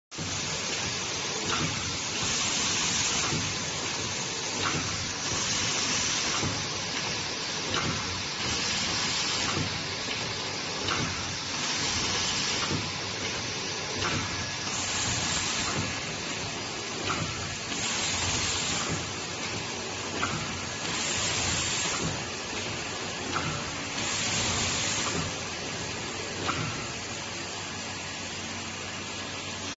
Single Color Glove Dotting Machine
GMT-23 is the latest model dual-function glove dotting machine. It can be used not only for adding TPR or Silica or Pvc to the palm side of the glove but also can be used for doing silk platform printing for the glove logo. The machine is with 20 pairs of hand molds and a high-power oven which can achieve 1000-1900 pairs/hour with three workers.